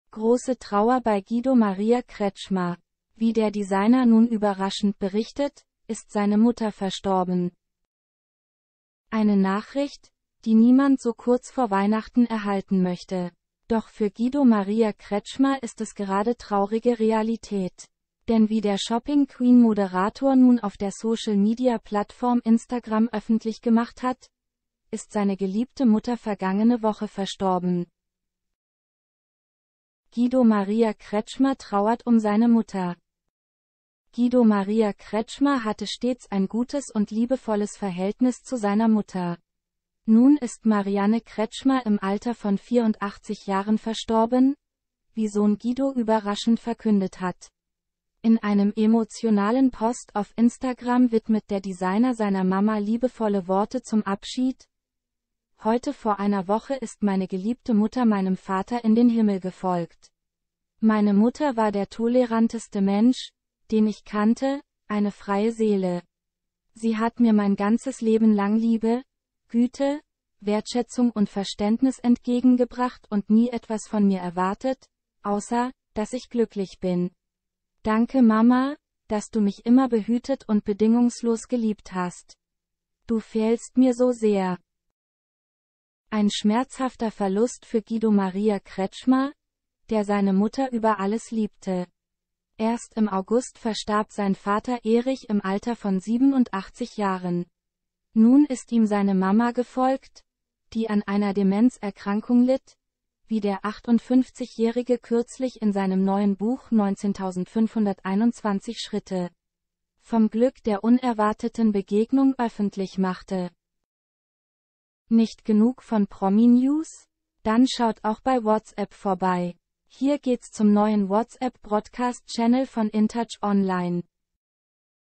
Guido Maria Kretschmer Bittere Neuigkeiten kurz vor Weihnachten! Seine Mutter ist gestorben
Große Trauer bei Guido Maria Kretschmer! Wie der Designer nun überraschend berichtet, ist seine Mutter verstorben.